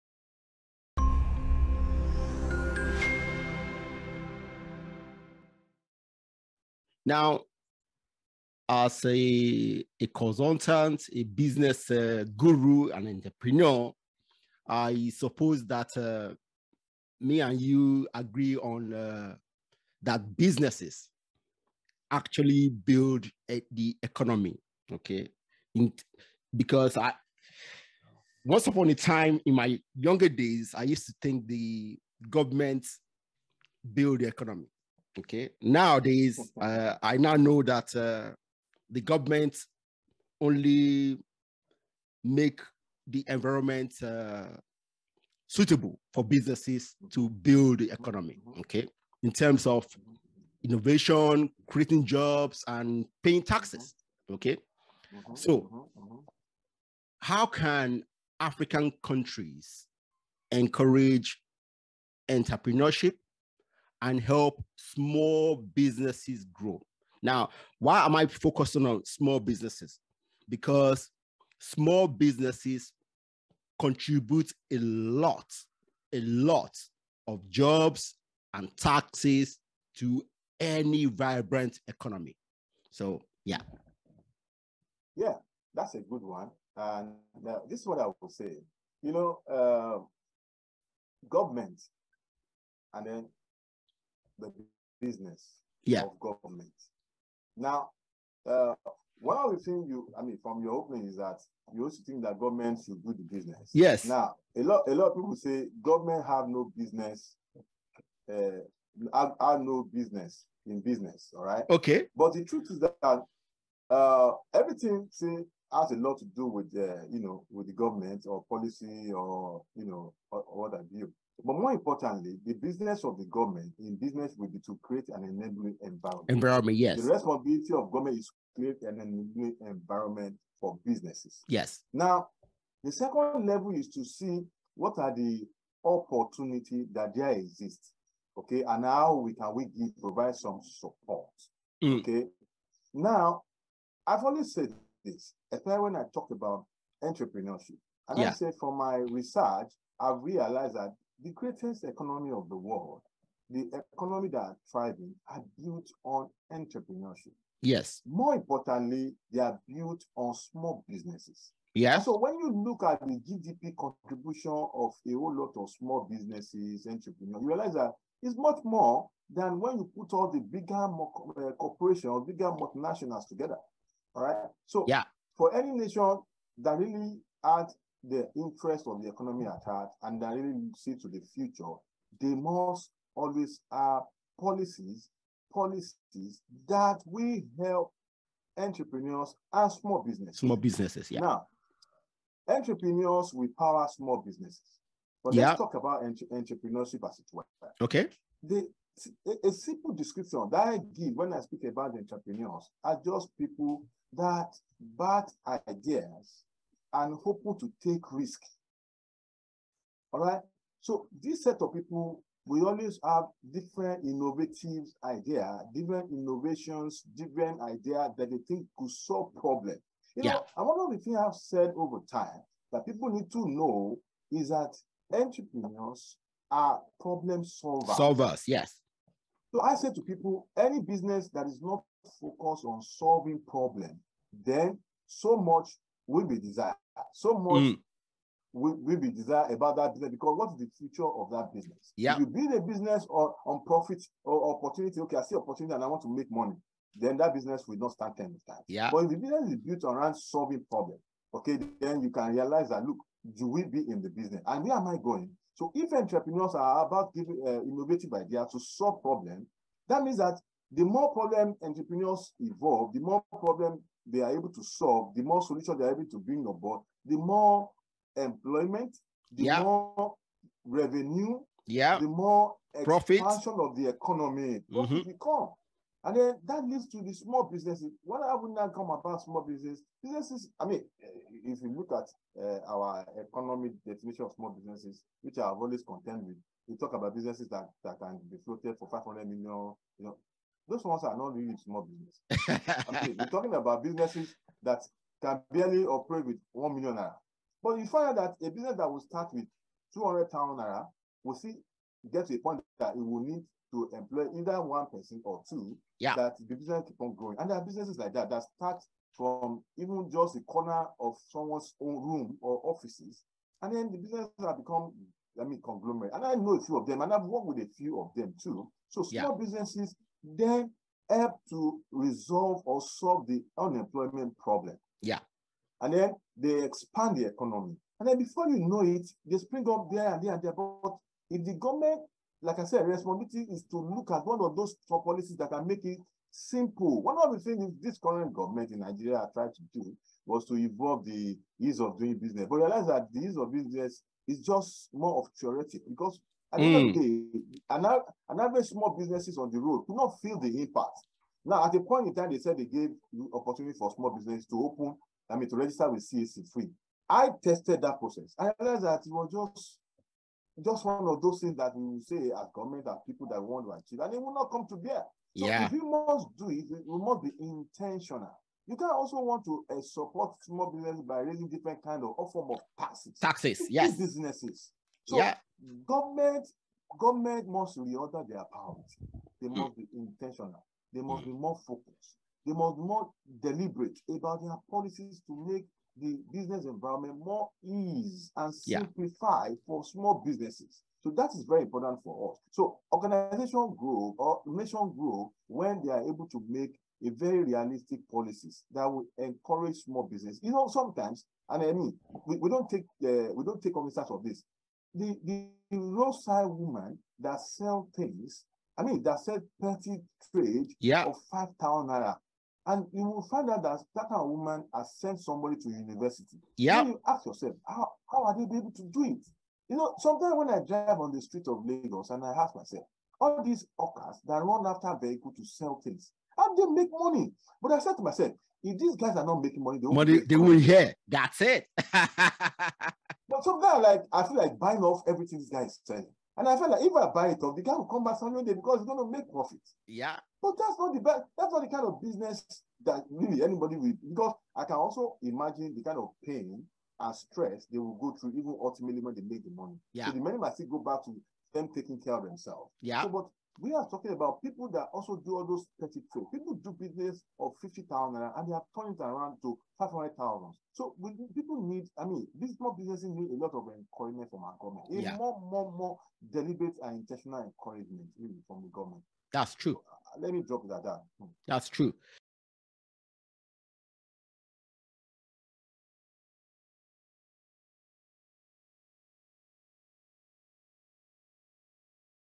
African Countries Need to Encourage Entrepreneurship and SMEs | Dr. Ayoade Adeyemi
In this episode of the Think BIG for Africa Podcast, my guest is Dr. Ayoade Adeyemi. Dr. Adeyemi is the CEO, of Lorache Consulting(HR & Marketing Consultancy). He is an international trainer and speaker, a Board member and Executive Director on a few organizations and a Governing Council member of a Nigerian Government-owned institution

In our conversation, we discussed Strategy, Productivity, Effectiveness, and Personal Values. Entrepreneurship, Small Business, Marketing, and Volunteerism